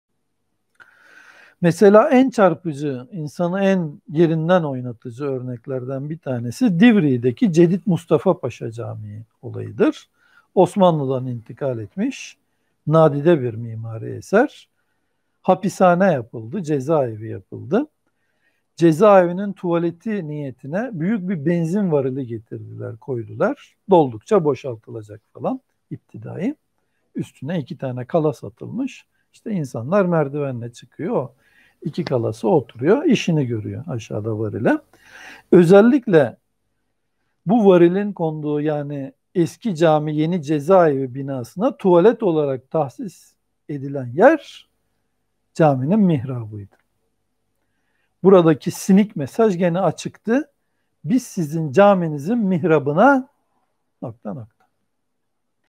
CAMİ MİHRABINI HELA YAPAN CHP NE DEMEK İSTEMİŞTİ?
Moğol ve Haçlı Seferleri'nde bile bunun bir benzerinin yaşandığı kayda geçmemiş. İslam Tarihi boyunca cami düşmanlığının bu derecesi sadece CHP/Tek Parti döneminde yaşandı.